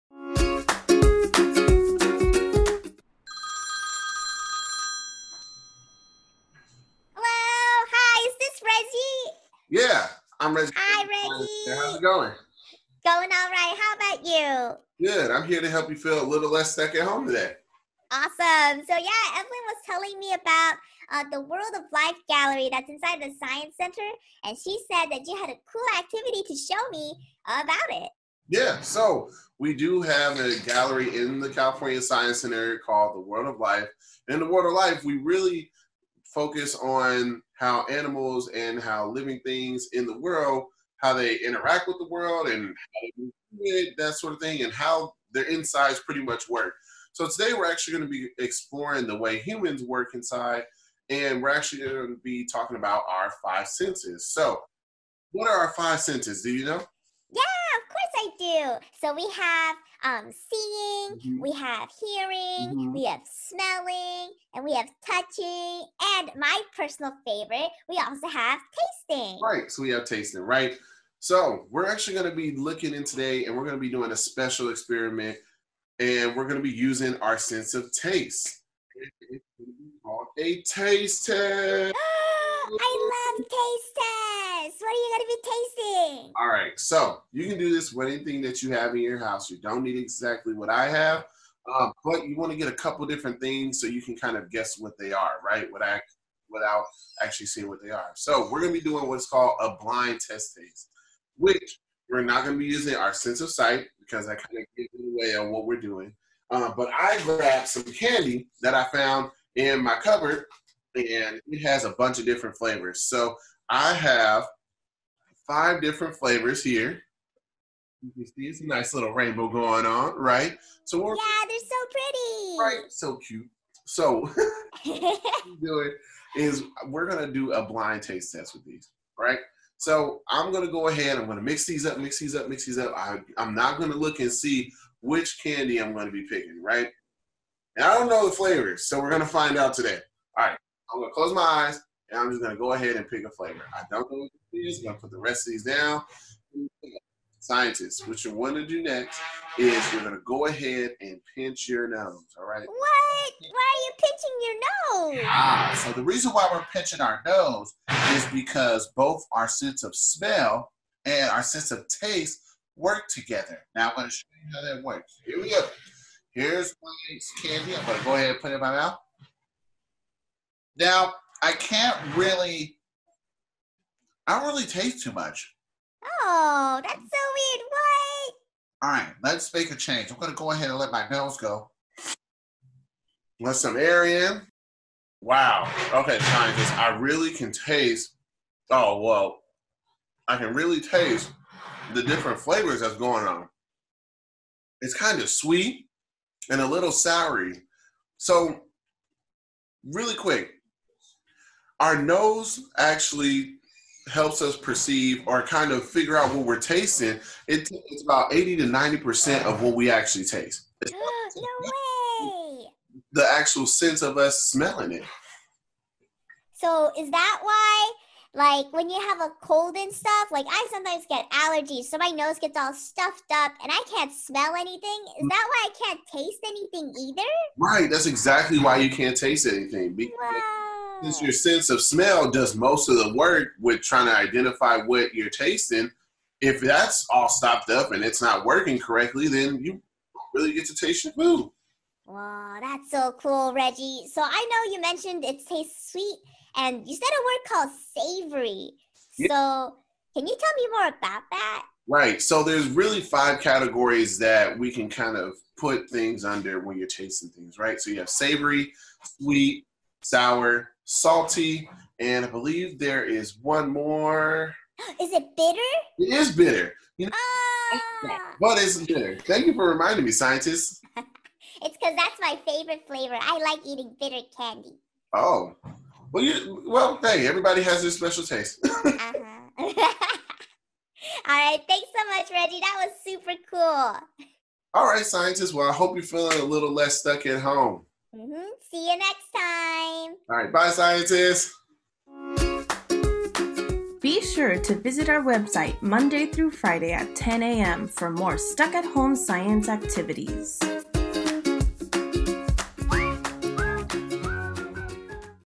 Tasty! - Stuck at Home Science | California Science Center
► What senses are we using when we eat? Are we tasting it or smelling it? Can it be both?
► Explore our World of Life exhibit themes! Join us May 18-22 and discover the connections between all life forms. From getting our hearts pumping to engineering ears, let's see how living things are alike. Let’s explore the World of Life from the comfort of your own home!

► ¿Qué sentidos usamos cuando comemos? ¿Lo estamos probando o oliendo? ¿Pueden ser las dos cosas?
► ¡Explora nuestros temas de exhibición de World of Life! Acompáñenos del 18 al 22 de mayo y descubra las conexiones entre todas las formas de vida. Desde conseguir que nuestros corazones bombeen, a construir orejas, veamos cómo los seres vivos son iguales. ¡Exploremos el mundo de la vida desde la comodidad de su propia casa!